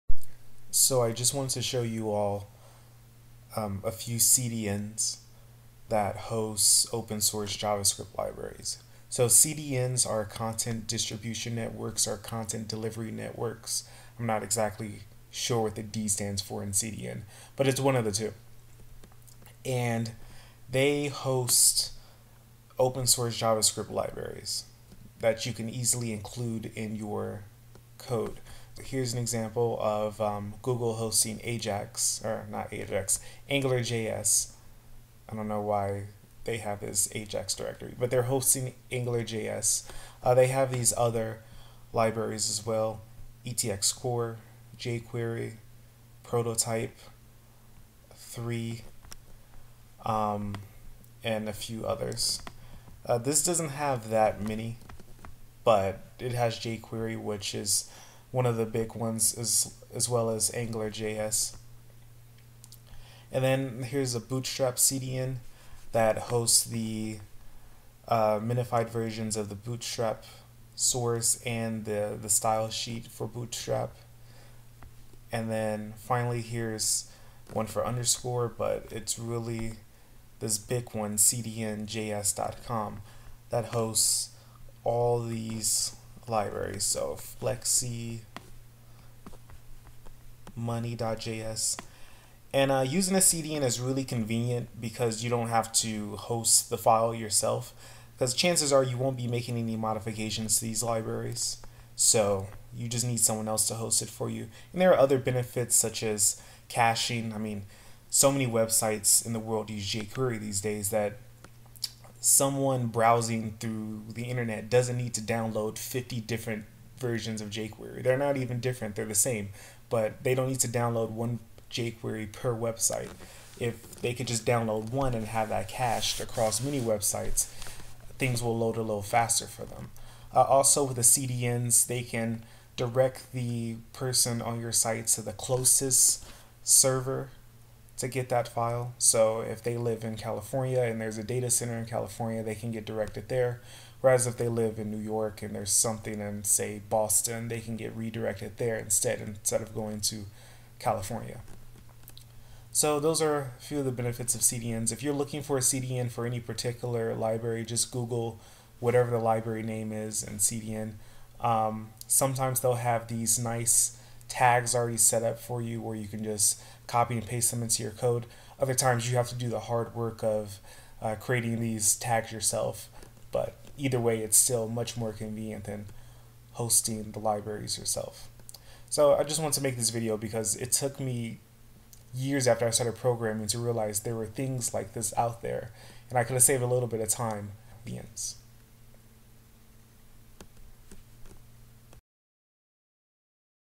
Using CDNs for JavaScript Library Hosting
This video discusses CDNs and demonstrates three sites for including open source libraries in your code.